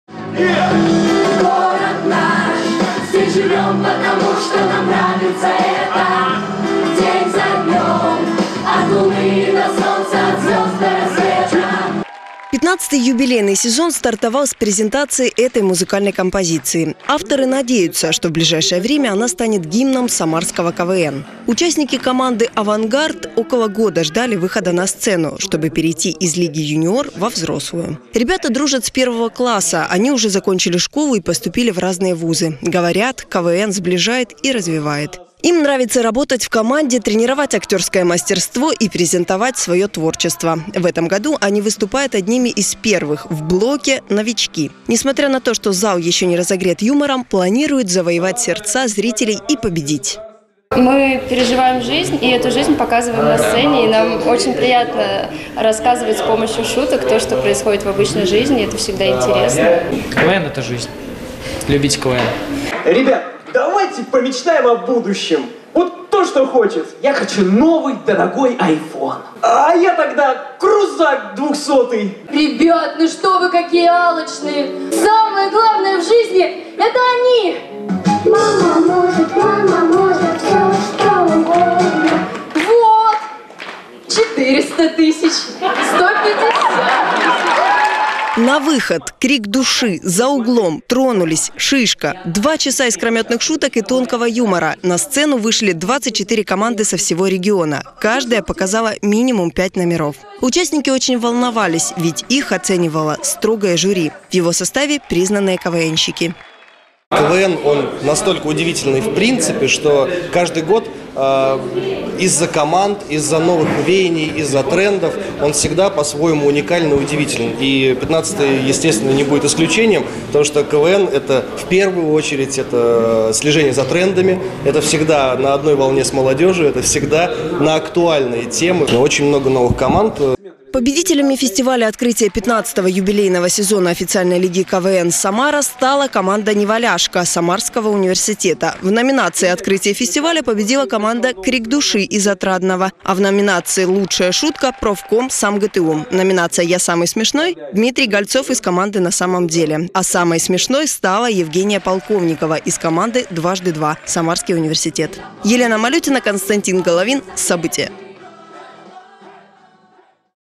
Стартовал 15-ый юбилейный сезон лиги КВН "Самара"
Хорошее настроение и море позитива.  В Самаре прошел фестиваль -открытие 15- го юбилейного сезона игр Официальной лиги КВН. На сцену вышли 24 команды со всего региона, каждая показала около 5 номеров. Участников оценивало строгое жюри. В  его составе  признанные КВНщики.Подробнее в сюжете Елены Малютиной.